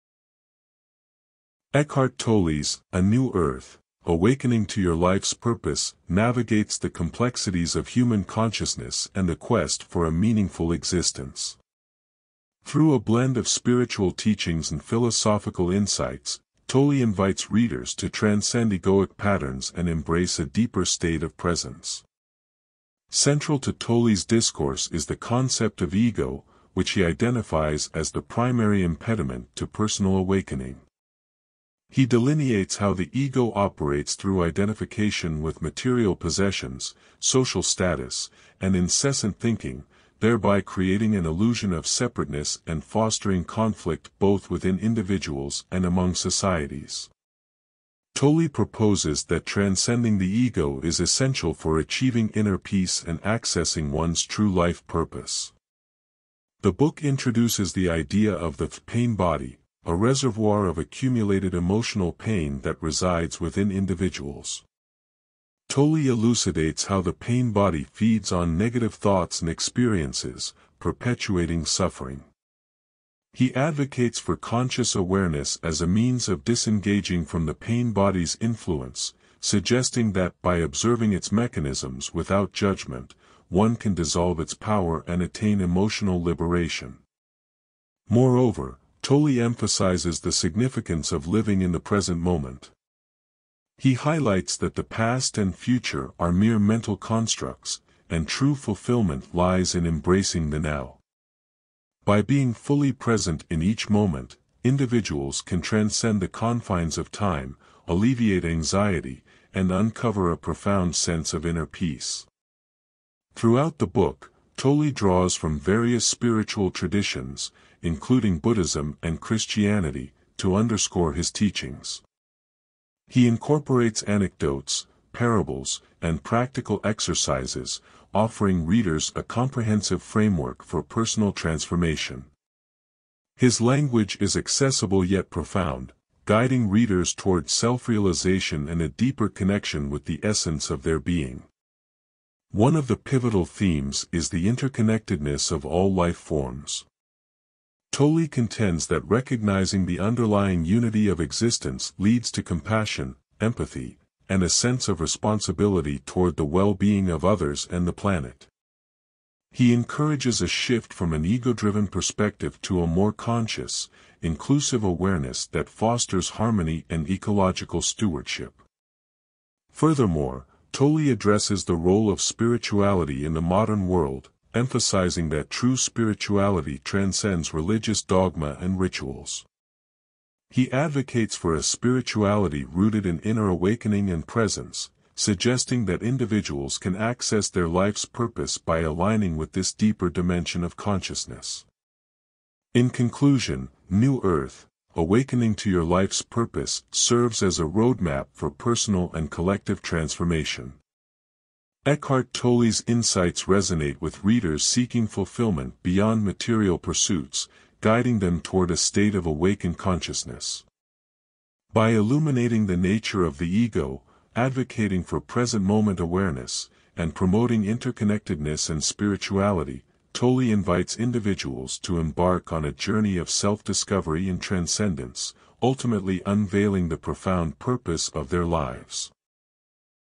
"A New Earth" By Eckhart Tolle
Eckhart Tolle's "A New Earth: Awakening to Your Life's Purpose" navigates the complexities of human consciousness and the quest for a meaningful existence. Through a blend of spiritual teachings and philosophical insights, Tolle invites readers to transcend egoic patterns and embrace a deeper state of presence.Central to Tolle's discourse is the concept of ego, which he identifies as the primary impediment to personal awakening. He delineates how the ego operates through identification with material possessions, social status, and incessant thinking, thereby creating an illusion of separateness and fostering conflict both within individuals and among societies. Tolle proposes that transcending the ego is essential for achieving inner peace and accessing one's true life purpose.The book introduces the idea of the "pain-body," a reservoir of accumulated emotional pain that resides within individuals. Tolle elucidates how the pain-body feeds on negative thoughts and experiences, perpetuating suffering. He advocates for conscious awareness as a means of disengaging from the pain-body's influence, suggesting that by observing its mechanisms without judgment, one can dissolve its power and attain emotional liberation.Moreover, Tolle emphasizes the significance of living in the present moment. He highlights that the past and future are mere mental constructs, and true fulfillment lies in embracing the Now. By being fully present in each moment, individuals can transcend the confines of time, alleviate anxiety, and uncover a profound sense of inner peace.Throughout the book, Tolle draws from various spiritual traditions, including Buddhism and Christianity, to underscore his teachings. He incorporates anecdotes, parables, and practical exercises, offering readers a comprehensive framework for personal transformation. His language is accessible yet profound, guiding readers toward self-realization and a deeper connection with the essence of their being.One of the pivotal themes is the interconnectedness of all life forms. Tolle contends that recognizing the underlying unity of existence leads to compassion, empathy, and a sense of responsibility toward the well-being of others and the planet. He encourages a shift from an ego-driven perspective to a more conscious, inclusive awareness that fosters harmony and ecological stewardship.Furthermore, Tolle addresses the role of spirituality in the modern world, emphasizing that true spirituality transcends religious dogma and rituals. He advocates for a spirituality rooted in inner awakening and presence, suggesting that individuals can access their life's purpose by aligning with this deeper dimension of consciousness.In conclusion, "A New Earth: Awakening to Your Life's Purpose" serves as a roadmap for personal and collective transformation. Eckhart Tolle's insights resonate with readers seeking fulfillment beyond material pursuits, guiding them toward a state of awakened consciousness. By illuminating the nature of the ego, advocating for present-moment awareness, and promoting interconnectedness and spirituality, Tolle invites individuals to embark on a journey of self-discovery and transcendence, ultimately unveiling the profound purpose of their lives.